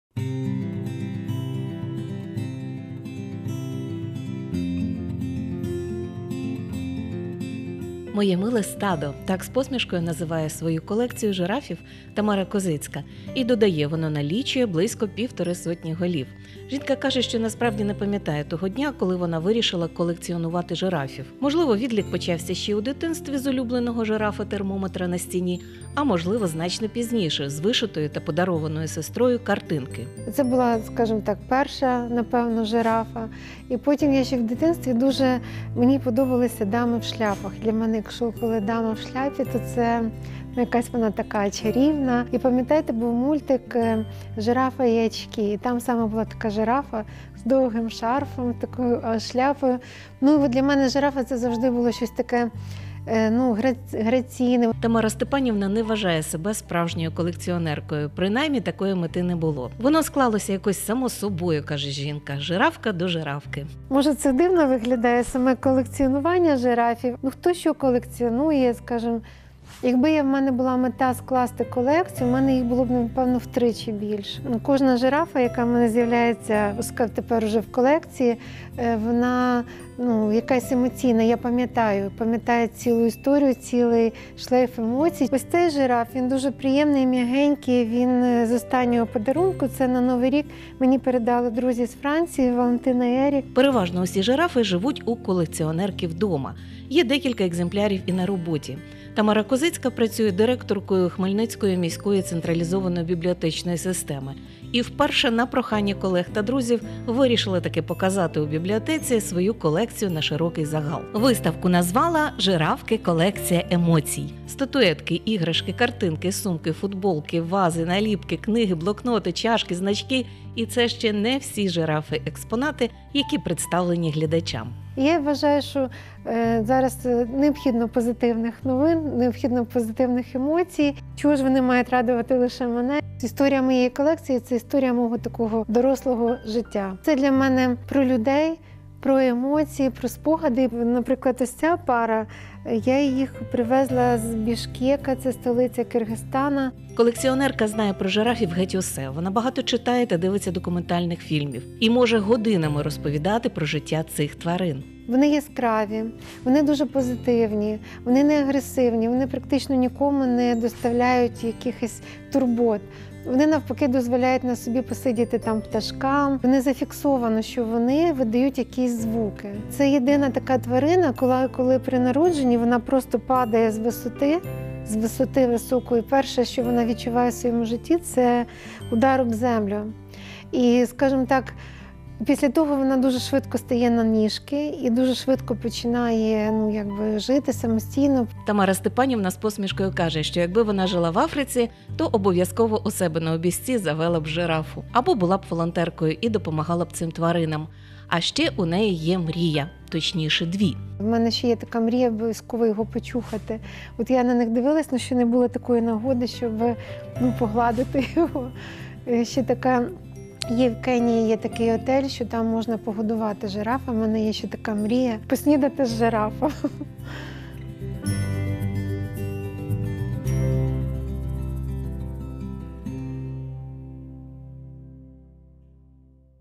Тамара Козицька - колекціонера жирафів I Ранок на Суспільному
«Моє миле стадо»,- так з посмішкою називає свою колекцію жирафів Тамара Козицька і додає : «Воно налічує близько півтори сотні голів». Жінка каже, що насправді  не пам'ятає того дня, коли вона вирішила колекціонувати жирафів. Можливо відлік почався ще у дитинстві з улюбленого жирафа-термометра на стіні, а можливо значно пізніше з вишитої  та  подарованої сестрою картинки.